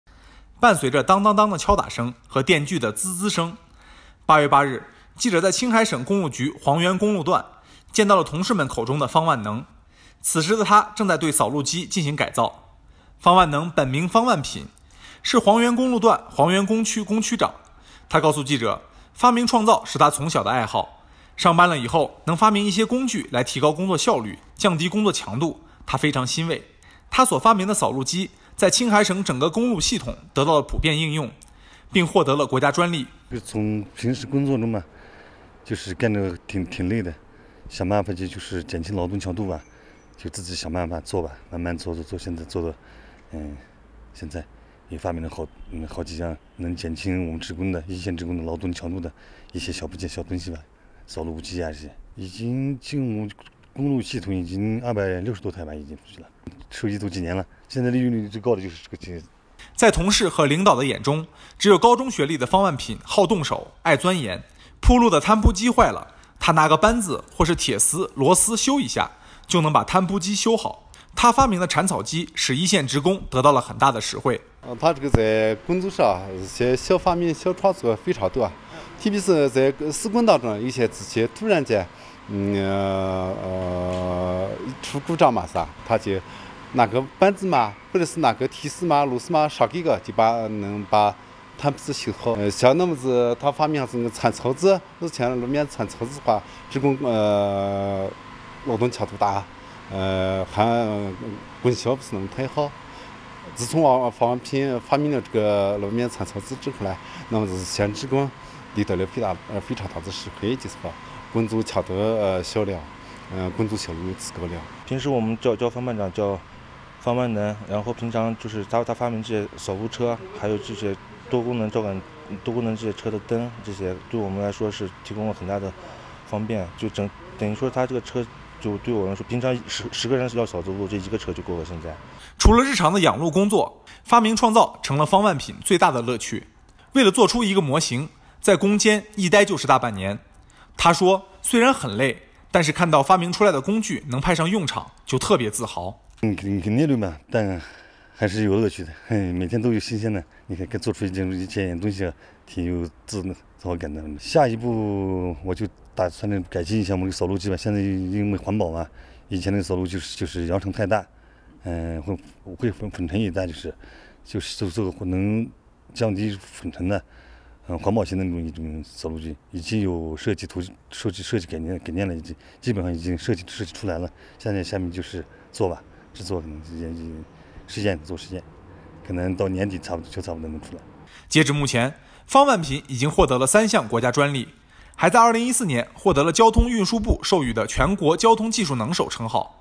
青海湟源养路工区里的发明家
伴随着“当当当”的敲打声和电锯的“滋滋”声，8月8日，记者在青海省公路局湟源公路段，见到了同事们口中的“方万能”，此时的他正在对扫路机进行改造。“方万能”本名方万品，是湟源公路段湟源工区工区长，他告诉记者，发明创造是他从小的爱好，上班了以后能发明一些工具来提高工作效率、降低工作强度，他非常欣慰。他所发明的扫路机，在青海省整个公路系统得到了普遍应用，并获得了国家专利。